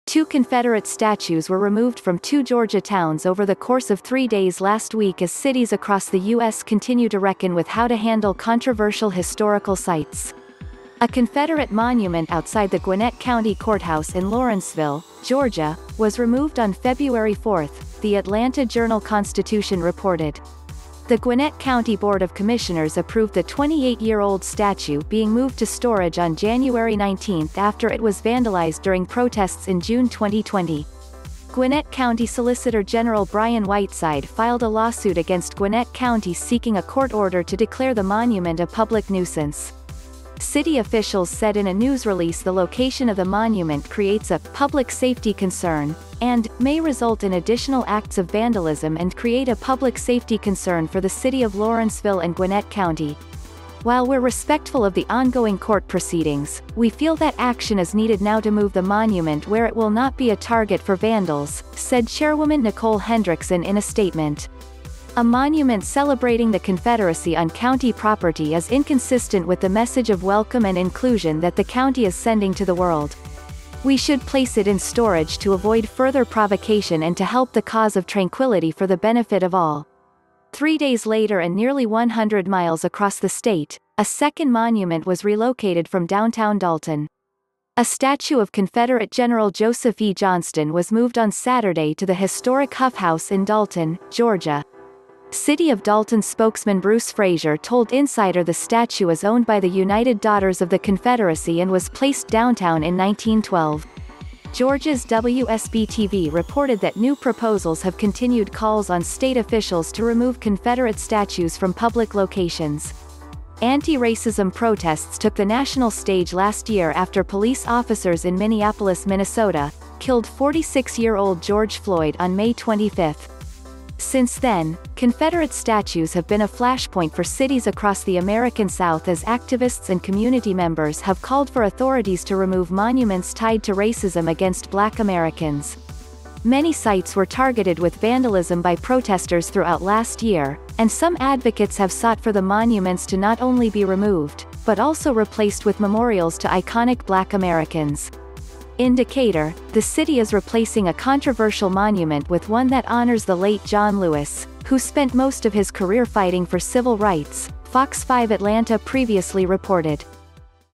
Georgia removed 2 Confederate statues in 1 week as cities continue to
Two Confederate statues were removed from two Georgia towns over the course of three days last week as cities across the US continue to reckon with how to handle controversial historical sites. A Confederate monument outside the Gwinnett County Courthouse in Lawrenceville, Georgia, was removed on February 4, the Atlanta Journal-Constitution reported. The Gwinnett County Board of Commissioners approved the 28-year-old statue being moved to storage on January 19 after it was vandalized during protests in June 2020. Gwinnett County Solicitor-General Brian Whiteside filed a lawsuit against Gwinnett County seeking a court order to declare the monument a public nuisance. City officials said in a news release the location of the monument creates a "public safety concern" and "may result in additional acts of vandalism and create a public safety concern for the City of Lawrenceville and Gwinnett County. ""While we're respectful of the ongoing court proceedings, we feel that action is needed now to move the monument where it will not be a target for vandals, " said Chairwoman Nicole Hendrickson in a statement. "A monument celebrating the Confederacy on county property is inconsistent with the message of welcome and inclusion that the County is sending to the world. We should place it in storage to avoid further provocation and to help the cause of tranquility for the benefit of all. "Three days later and nearly 100 miles across the state, a second monument was relocated from downtown Dalton. A statue of Confederate General Joseph E. Johnston was moved on Saturday to the historic Huff House in Dalton, Georgia. City of Dalton spokesman Bruce Frazier told Insider the statue is owned by the United Daughters of the Confederacy and was placed downtown in 1912. Georgia's WSB-TV reported that new proposals have continued calls on state officials to remove Confederate statues from public locations. Anti-racism protests took the national stage last year after police officers in Minneapolis, Minnesota, killed 46-year-old George Floyd on May 25. Since then, confederate statues have been a flashpoint for cities across the American South as activists and community members have called for authorities to remove monuments tied to racism against Black Americans. Many sites were targeted with vandalism by protesters throughout last year, and some advocates have sought for the monuments to not only be removed, but also replaced with memorials to iconic Black Americans. In Decatur, the city is replacing a controversial monument with one that honors the late John Lewis, who spent most of his career fighting for civil rights, FOX 5 Atlanta previously reported.